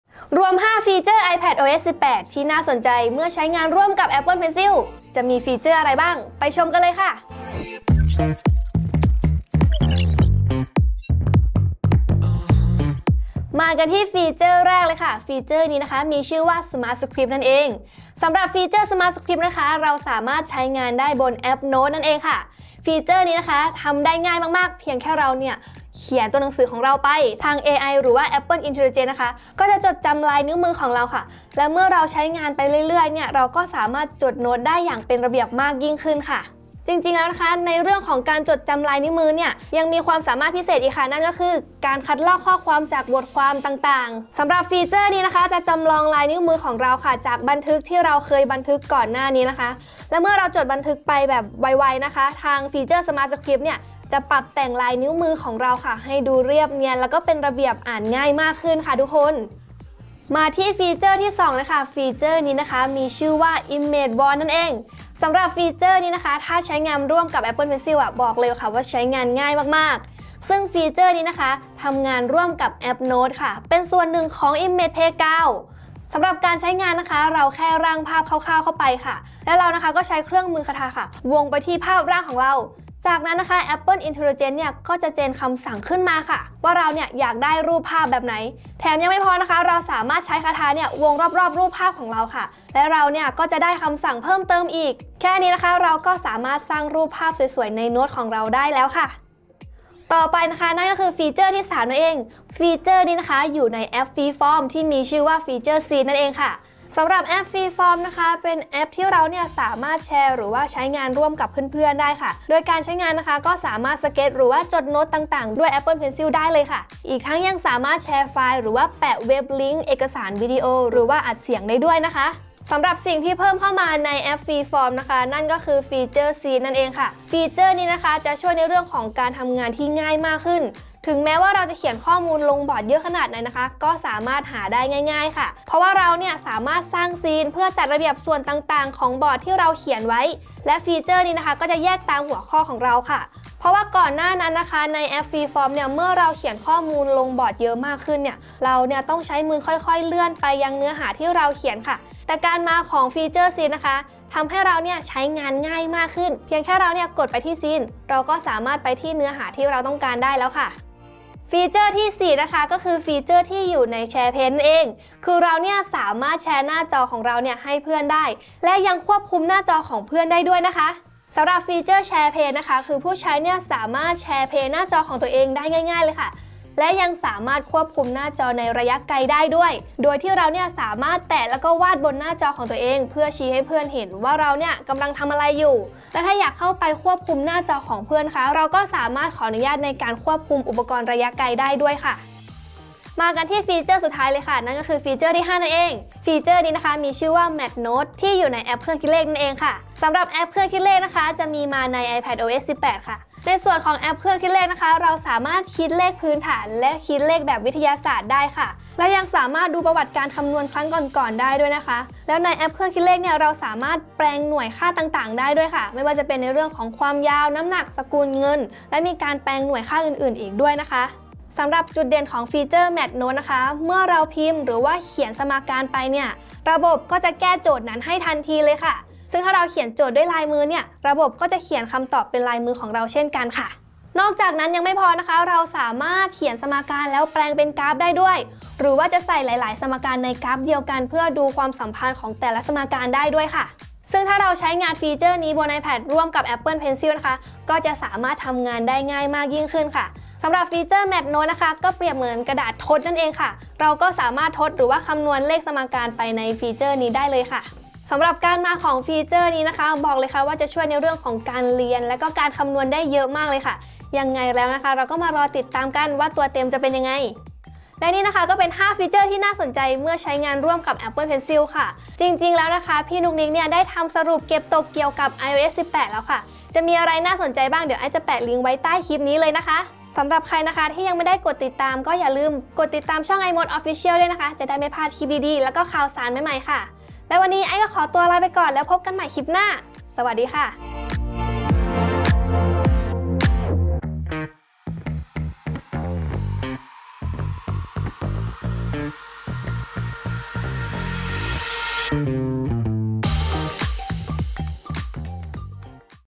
รวม 5 ฟีเจอร์ iPadOS 18 น่าสนใจ ที่ใช้งานกับ Apple Pencil
Apple เปิดตัว iPadOS 18 มาพร้อมฟีเจอร์น่าสนใจมากมาย และบางฟีเจอร์ใช้งานร่วมกับ Apple Pencil ได้ด้วย จะมีอะไรบ้าง ชมสรุปได้เลย

00:00 - ฟีเจอร์ iPadOS 18 ที่ทำงานร่วมกับ Apple Pencil
00:13 - Smart Script
00:59 - Image Wand
01:39 - Scene in Freeform
02:41 - SharePlay
03:12 - Math Note
04:27 - สรุป

📌 คลิปที่น่าสนใจ

เก็บตก 18 ฟีเจอร์ iOS 18 น่าสนใจ ที่ Apple ไม่ได้โชว์ในงาน WWDC24

สรุปเปิดตัว iOS 18, iPadOS 18, macOS Sequoia ของใหม่เพียบ น่าใช้สุดๆ

สรุปอุปกรณ์รุ่นรองรับ iOS 18, iPadOS 18, watchOS 11 และ macOS Sequoia